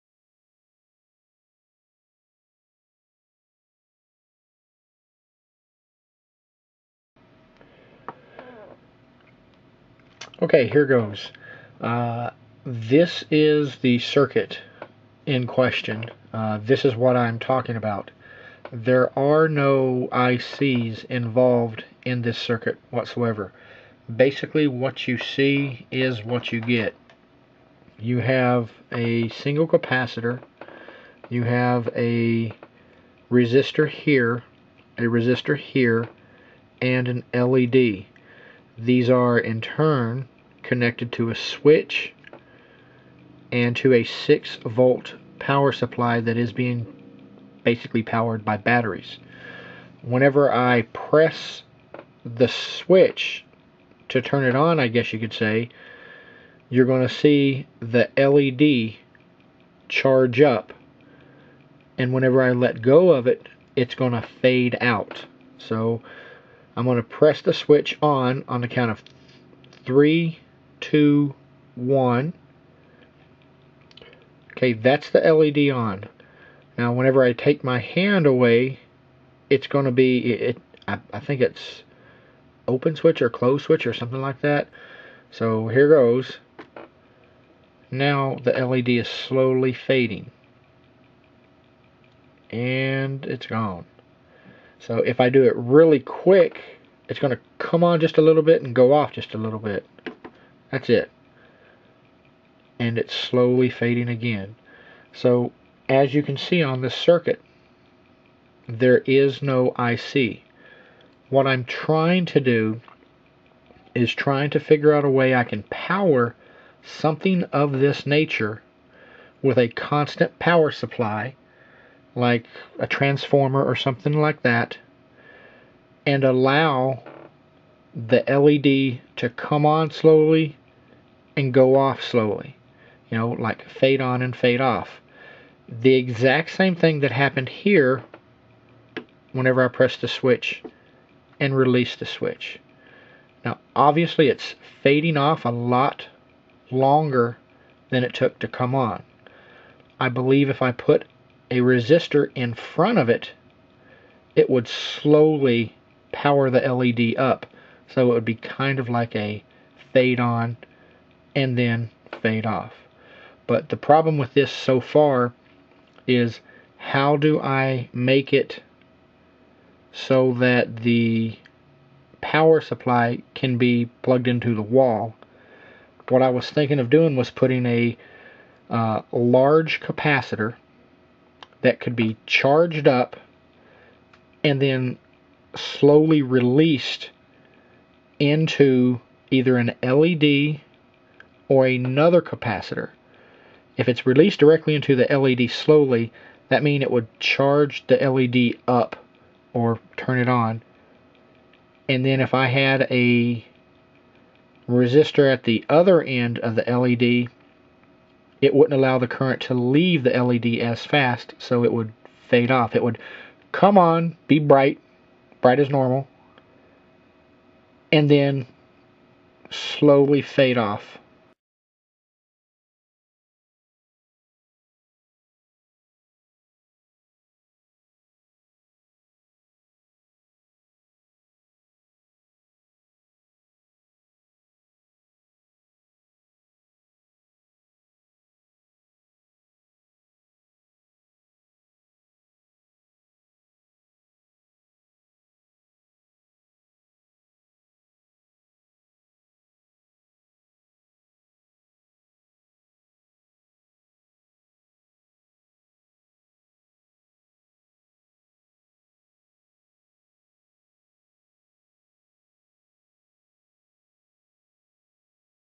How do I make a LED FADE without an IC
This is an LED FADING OUT without a 555 IC being used.  This entire "unit" ONLY has in use 2 Resistors, 1 capacitor, 1 LED, and one switch.

I am TRYING to figure out HOW to make the LED FADE in, then FADE out without using a 555 (or ANY IC) Or for this matter IF it can even be done.

 Here I SEE IT, being done by using a switch and me manually "turning it on"  but can it be done with a constant power source.